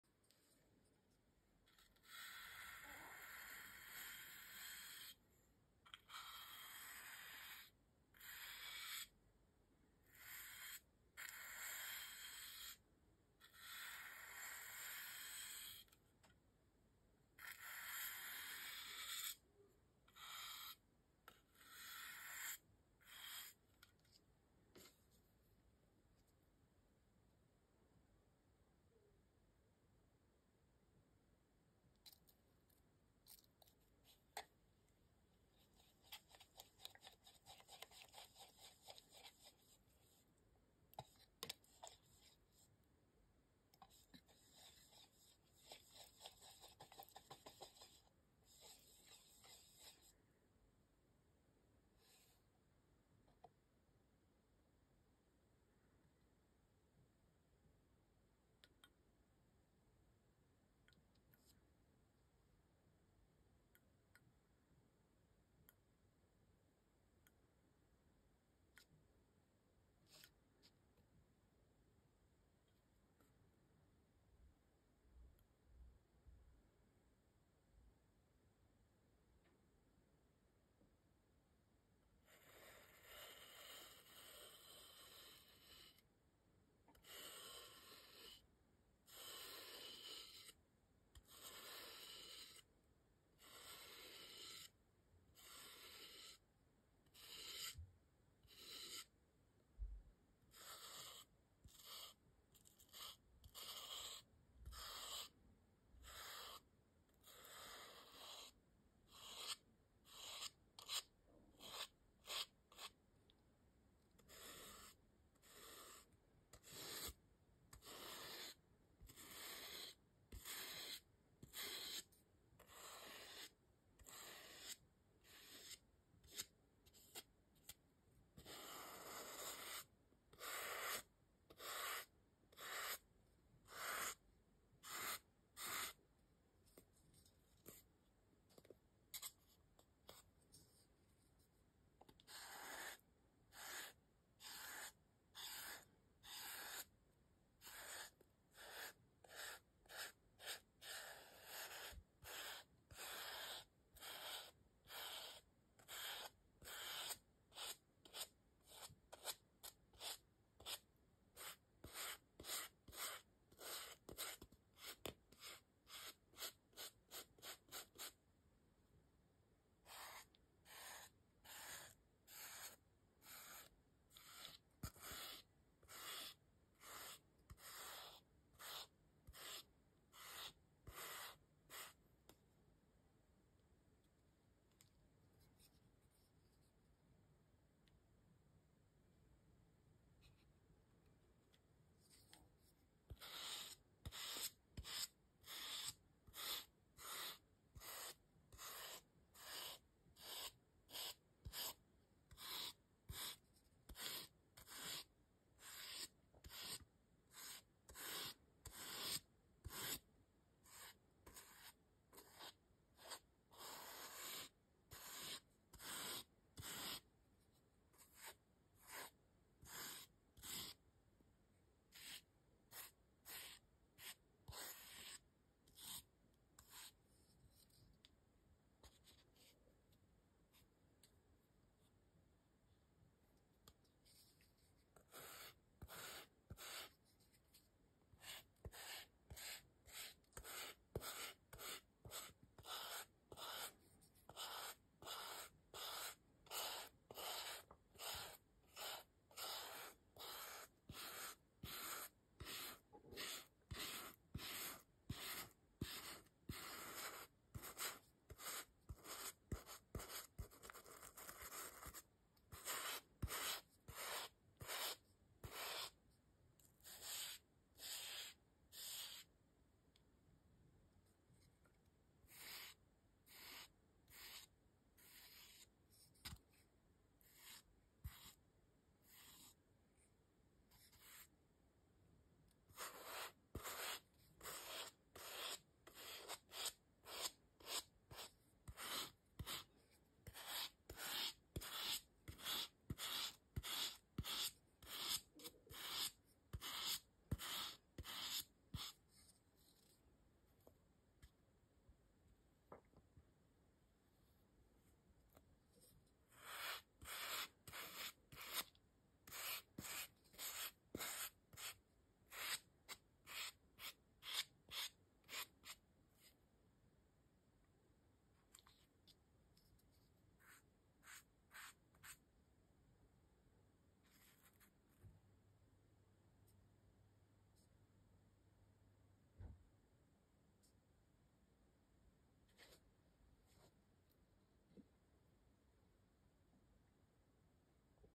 Styrofoam coloring „a Star is born“
Einen Styropor Stern mit Goldfarbe bemalt.

Gefilmt mit meinem Iphone.